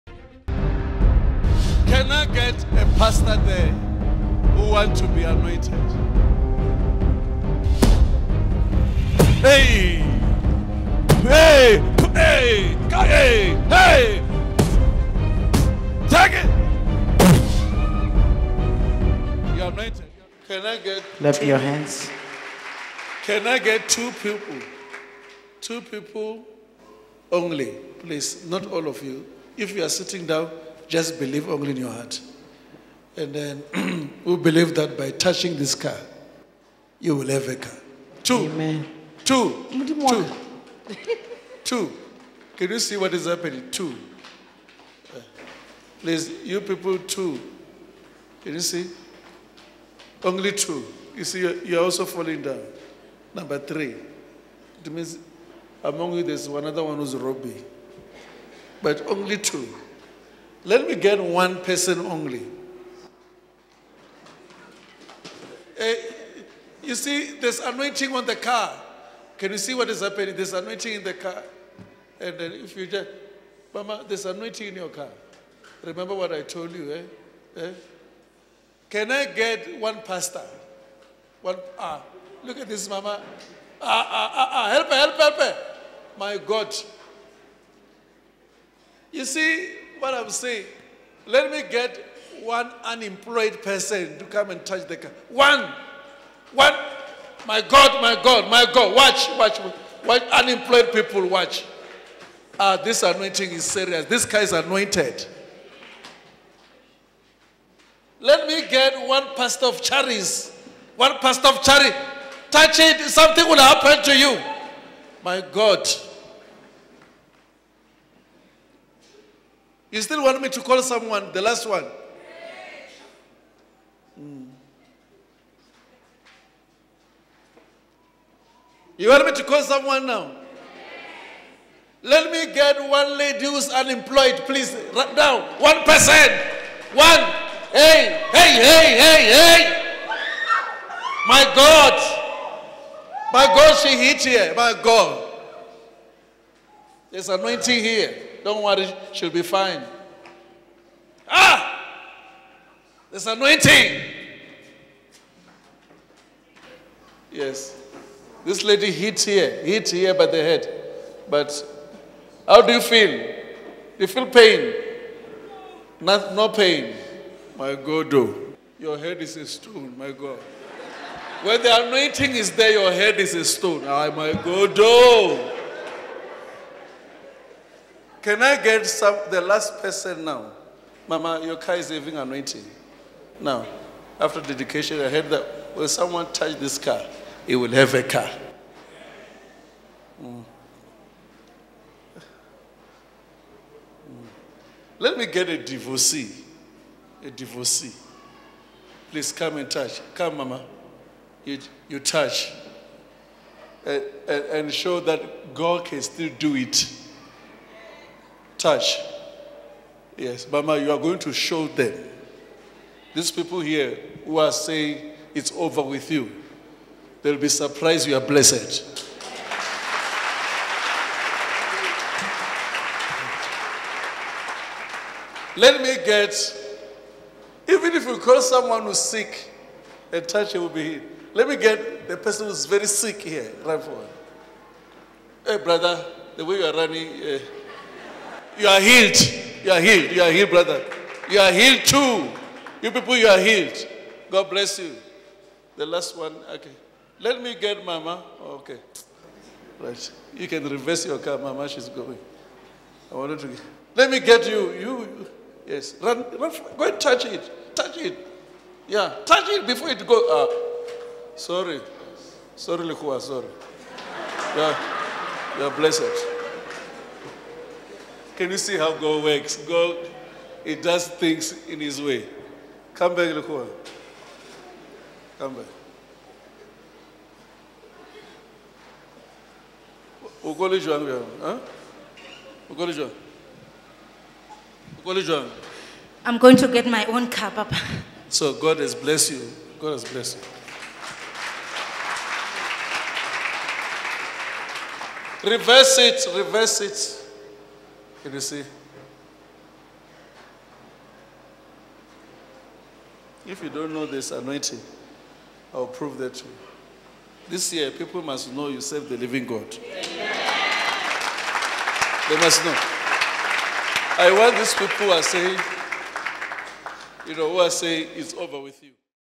God does His things His way….ANOINTED CAR used to MINISTER to the Congregants
During the dedication of a car testified by one woman, Apostle JB Makananisa calls out various congregants to come out by faith and touch the Anointed Car to receive their own cars.